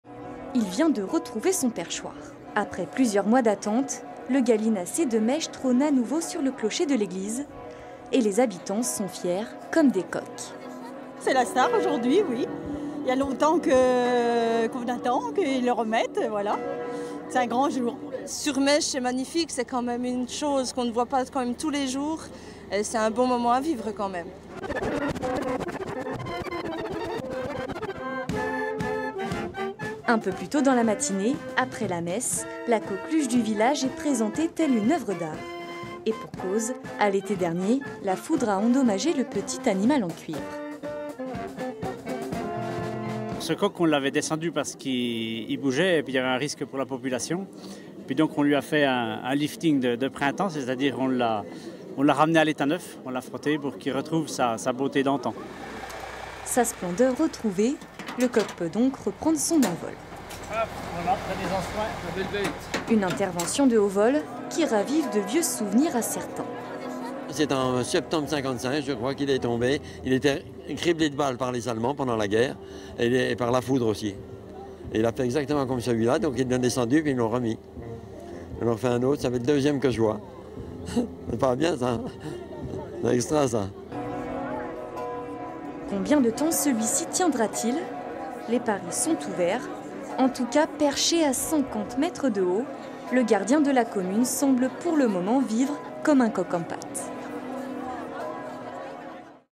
Maîche : le coq touché par la foudre a retrouvé le clocher de l'église
Avec : Jean-Michel Feuvrier -  Adjoint à Maîche en charge de l'urbanisme.

Un reportage de Mary Sohier, Maxime Meuneveaux et Manu Blanc.